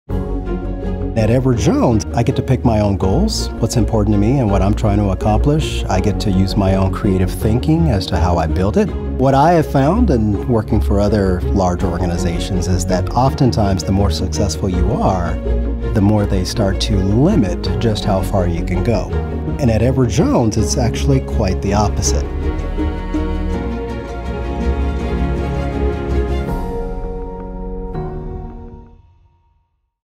Meet the Edward Jones Team| Edward Jones Careers
Meet Edward Jones. Hear from our financial advisors on how the autonomy, resources, and support empower at Edward Jones empowers them to make more than just a living.